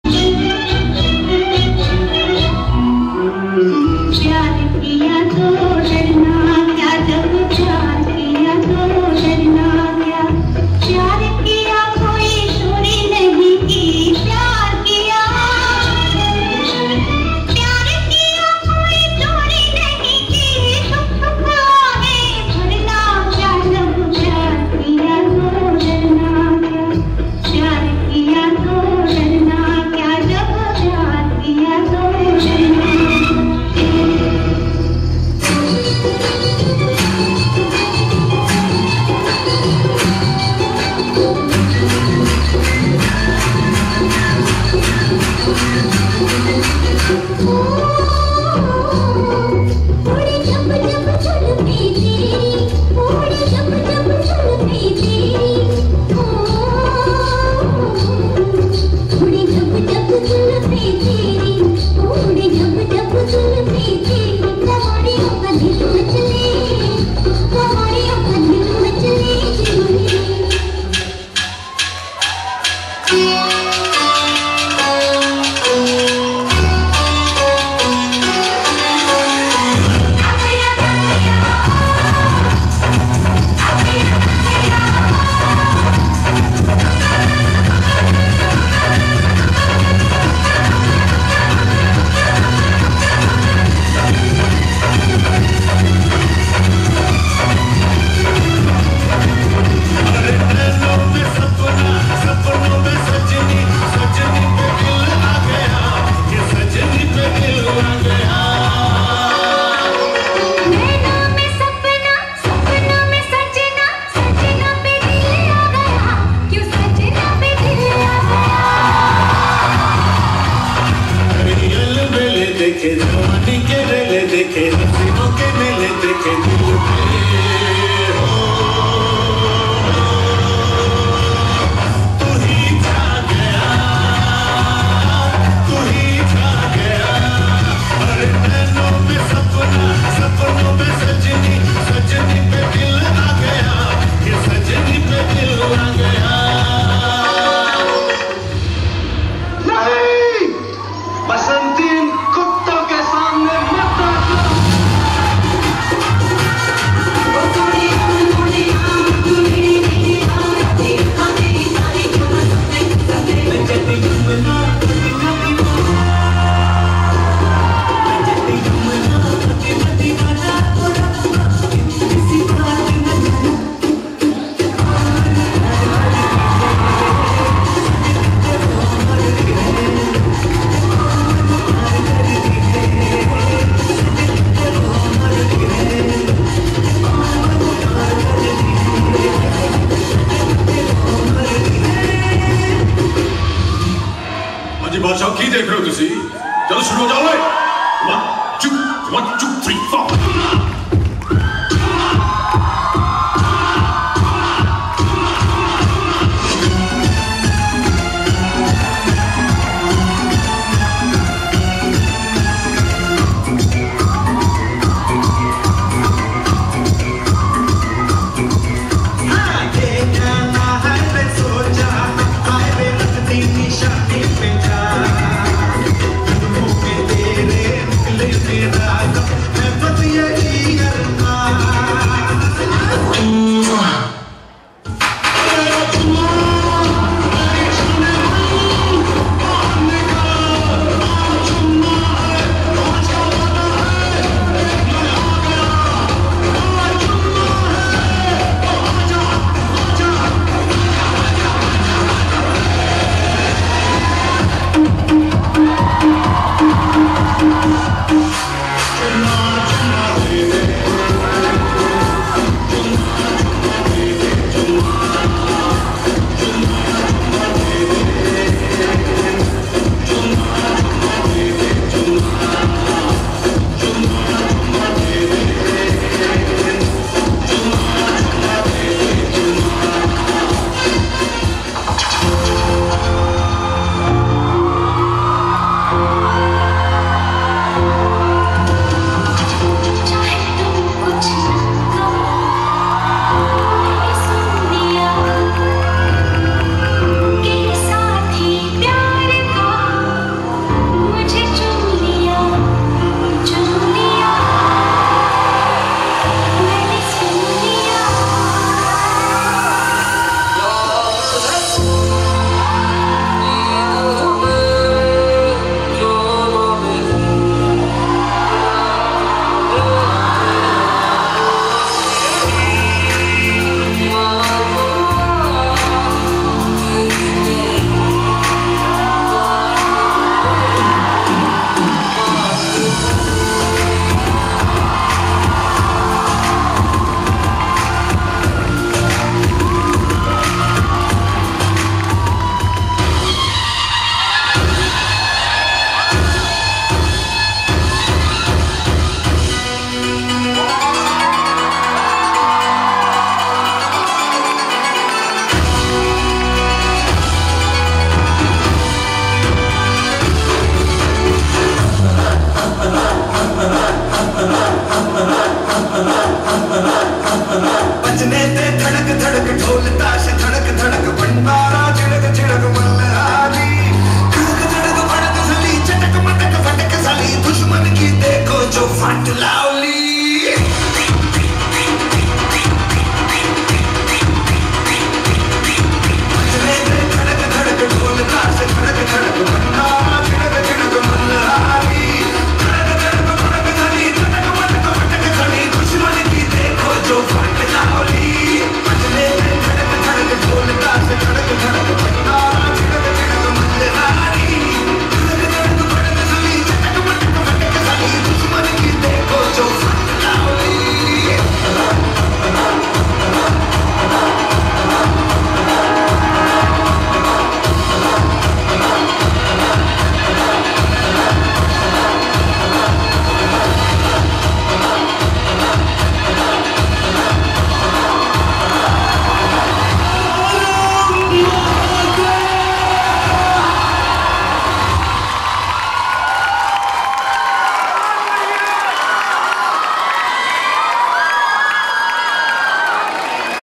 Bollywood Through The years - MGIMS Medical Students
Intercollegiate Dance Competition at NKP Salve Medical College, Nagpur on Feb 2016.. 1st position won by MGIMS Sevagram.